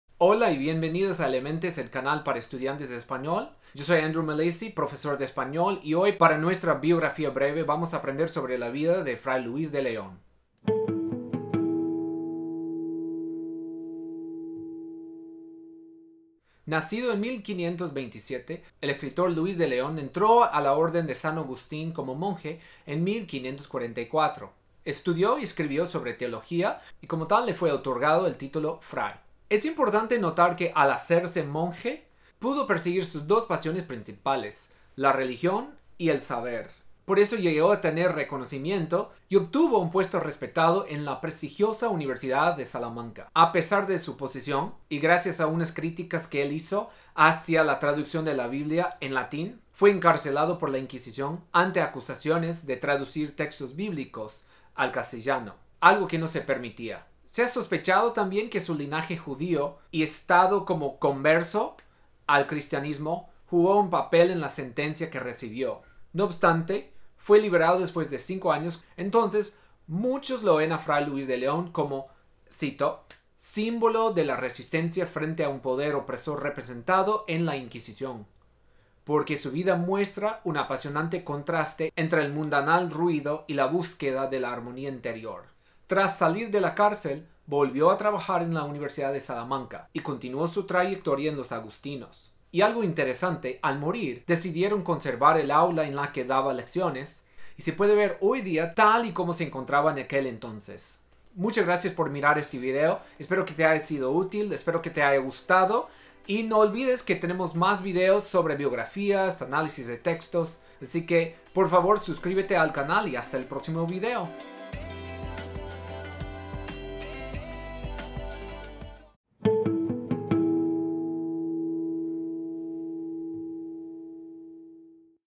Fray Luis de León | Biografía breve
Biografía breve de Fray Luis de León. No olvides de subscribirte. ¡Deja tus comentarios y preguntas abajo!

Texto completo:
Por Andrew Milacci
Fray Luis de León

Nacido en 1527, el escritor Luis de León entró en la orden de San Agustín como monje en 1544, estudió y escribió sobre teología, y como tal le fue otorgado el título “Fray”. Murió en 1591.

Es importante notar que, al hacerse monje, pudo perseguir sus dos pasiones principales: la religión y el saber. Por eso, llegó a tener reconocimiento y obtuvo un puesto respetado en la prestigiosa Universidad de Salamanca. A pesar de su posición, y gracias a unas críticas que él hizo hacia la traducción de la Biblia en latín, fue encarcelado por la Inquisición ante acusaciones de traducir textos bíblicos al castellano, algo que no se permitía. Se ha sospechado, asimismo, que su linaje judío y estado como “converso” al cristianismo jugó un papel en la sentencia que recibió.

No obstante, fue liberado después de 5 años. Entonces, muchos lo ven a Fray Luis de León como “símbolo de la resistencia frente a un poder opresor representado en la Inquisición, [porque] su vida muestra un apasionante contraste entre el mundanal ruido y la búsqueda de la armonía interior”.

Tras salir de la cárcel, volvió a trabajar en la Universidad de Salamanca y continuó su trayectoria en los agustinos. Y algo interesante, al morir, decidieron conservar el aula en la que daba lecciones, y se puede ver hoy día tal y como se encontraba en aquel entonces.

Friedman, Valdivieso, Virgillo. “Aproximaciones al estudio de la literatura hispánica”. McGraw Hill.
Mayne-Kienzle, Méndez-Faith, Vetterling. “Panoramas literarios: España”. 2nd ed. Cengage. 2013.